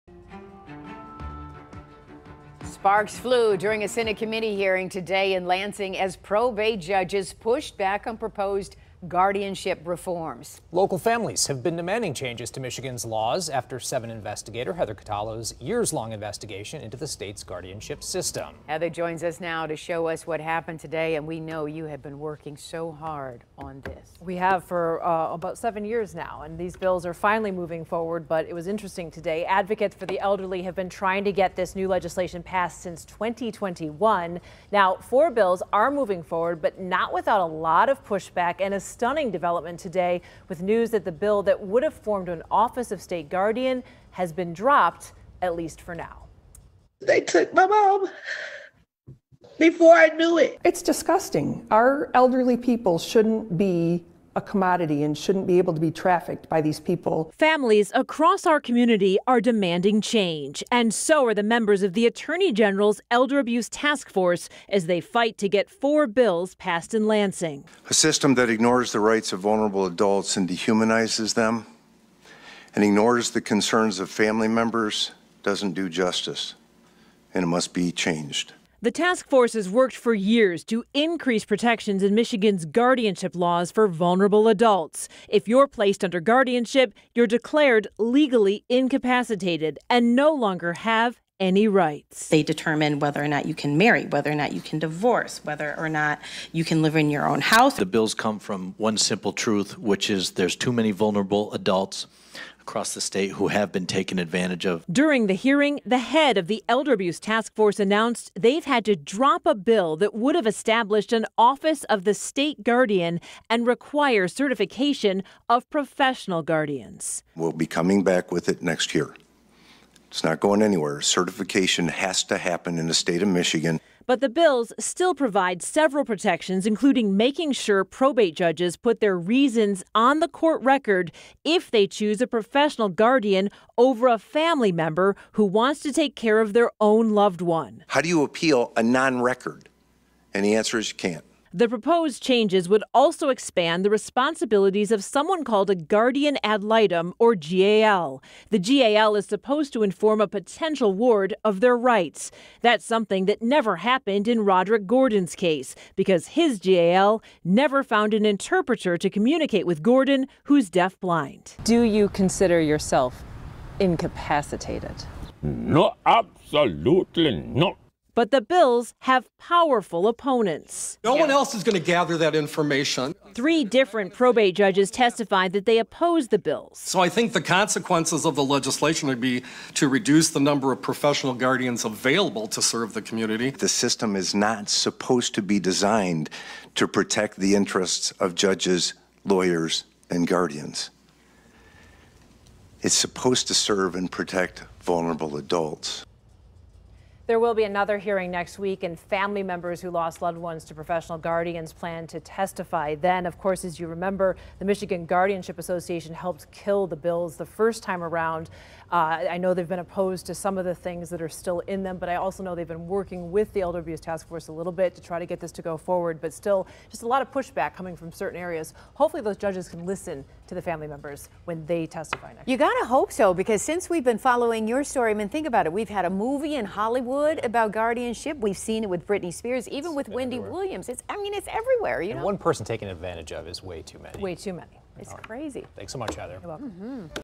Senate hearing on new guardianship reform bills turns contentious
A hearing on bills that would reform Michigan's guardianship laws was held in Lansing at the State Senate with testimony revealing that some of the measures had been dropped and would be brought back up next term.